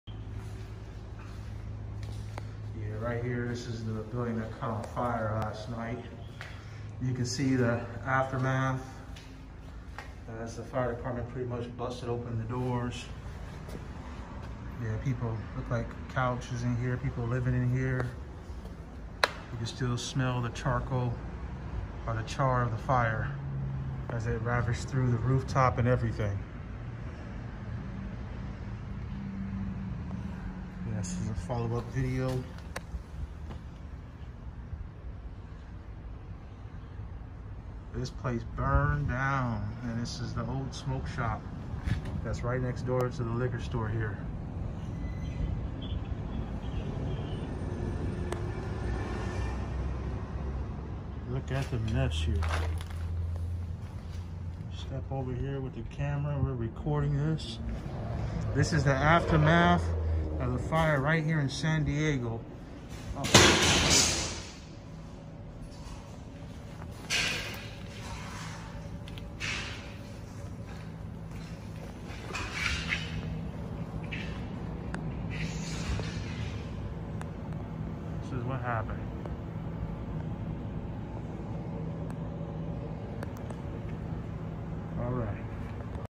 Aftermath of the fire last night
July 17, 2020 fire broke out between The Boulevard Wine & Spirits located at 4245 El Cajon Blvd, San Diego, CA 92105 and another building next door which maybe a smoke shop 4247 El Cajon Blvd, San Diego CA 92105

Aftermath of the fire that burned down the building next-door luckily you didn’t do no damage to the liquor store next-door